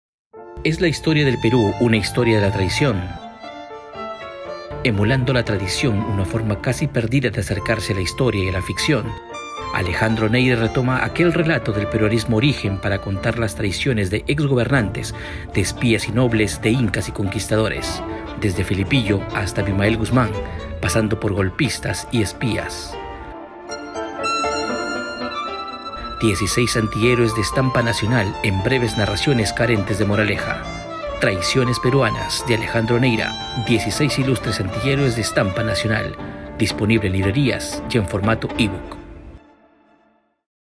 Locución para la venta del libro Traiciones peruanas
Locución para la venta del libro Traiciones peruanas de Alejandro Neyra  por encargo de Penguin Randon House para la emisión en el programa No Hay Derecho de Ideeleradio.